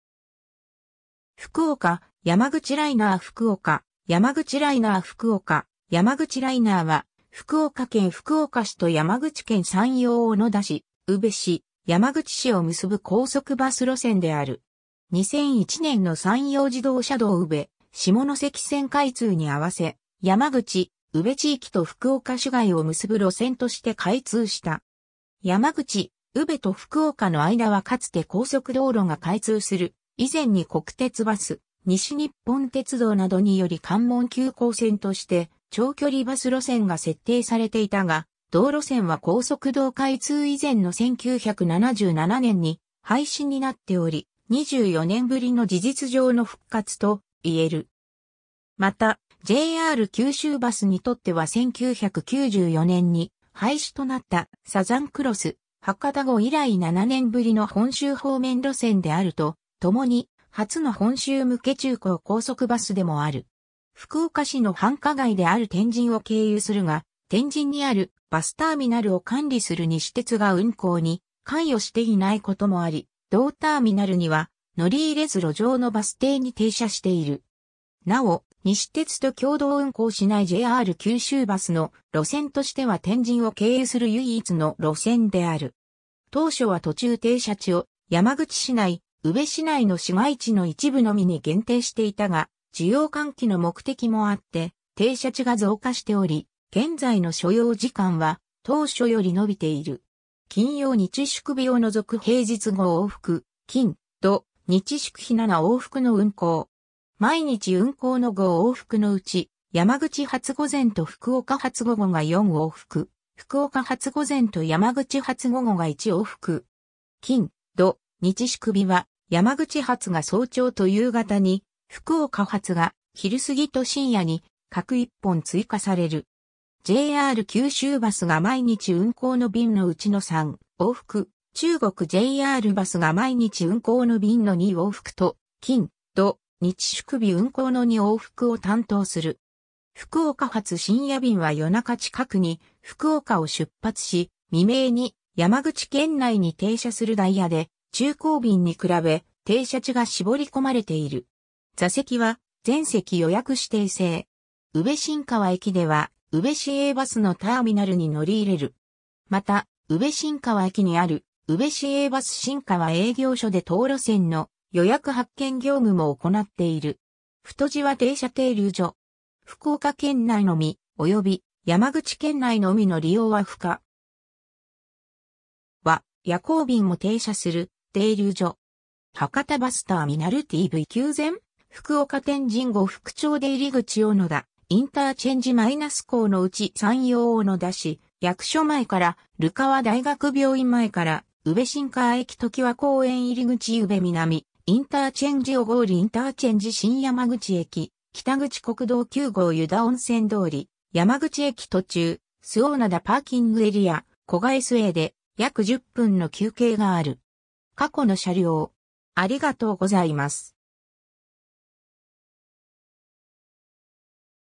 福岡・山口ライナー
福岡・山口ライナー（中国JRバス） 福岡・山口ライナー（JR九州バス） 福岡・山口ライナー（ふくおか・やまぐちライナー）は、福岡県福岡市と山口県山陽小野田市・宇部市・山口市を結ぶ高速バス路線である。
 2001年の山陽自動車道宇部下関線開通にあわせ、山口・宇部地域と福岡市街を結ぶ路線として開通した。
山口・宇部と福岡の間はかつて高速道路が開通する以前に国鉄バス・西日本鉄道（西鉄）などにより関門急行線として長距離バス路線が設定されていたが、同路線は高速道開通以前の1977年に廃止になっており、24年ぶりの事実上の復活といえる。
また、JR九州バス（前身のJR九州時代を含む）にとっては1994年に廃止となったサザンクロス博多号以来7年ぶりの本州方面路線であるとともに、初の本州向け昼行高速バスでもある。
 福岡市の繁華街である天神を経由するが、天神にあるバスターミナル（西鉄天神高速バスターミナル）を管理する西鉄が運行に関与していないこともあり、同ターミナルには乗り入れず路上のバス停に停車している（同様の停車形態を取るのは他に昭和自動車のいと・しま号がある）。
なお、西鉄と共同運行しないJR九州バスの路線としては天神を経由する唯一の路線である。
 当初は途中停車地を山口市内・宇部市内の市街地の一部のみに限定していたが、需要喚起の目的もあって停車地が増加しており、現在の所要時間は当初より延びている。
 金曜日・祝日を除く平日5往復、金・土・日・祝日7往復の運行。
毎日運行の5往復のうち、山口発午前と福岡発午後が4往復、福岡発午前と山口発午後が1往復。
金・土・日・祝日は山口発が早朝と夕方に、福岡発が昼過ぎと深夜に各1本追加される。
JR九州バスが毎日運行の便のうちの3往復、中国JRバスが毎日運行の便の2往復と金・土・日・祝日運行の2往復を担当する。
福岡発深夜便は夜中近くに福岡を出発し未明に山口県内に停車するダイヤで、昼行便に比べ、停車地が絞り込まれている。
 座席は全席予約指定制。
 宇部新川駅では宇部市営バスのターミナルに乗り入れる。
また、宇部新川駅にある宇部市営バス新川営業所（きっぷ売り場）で当路線の予約発券業務も行っている。
 太字は停車停留所。
福岡県内のみ、および山口県内のみの利用（福岡天神 - 江の内間を通らない利用）は不可。
★は夜行便も停車する停留所。
 博多バスターミナル★ - TVQ前★ - 福岡天神★ - 呉服町出入口 - （福岡高速環状線・1号香椎線・4号粕屋線・九州自動車道・関門橋・中国自動車道・山陽自動車道宇部下関線） - 小野田IC - 江の内★ - 山陽小野田市役所前 - 流川 - （国道190号） - 大学病院前 - 宇部新川駅★- ときわ公園入口 - （国道190号） - 宇部南IC - （山口宇部道路・小郡道路） - 小郡IC (小郡道路) - （国道9号） - 新山口駅北口★- 国道9号） - （山口県道204号宮野大歳線） - 湯田温泉通★ - （山口県道204号宮野大歳線） - 山口駅★ ※途中、周防灘PA・古賀SAで約10分の休憩がある。
 過去の車両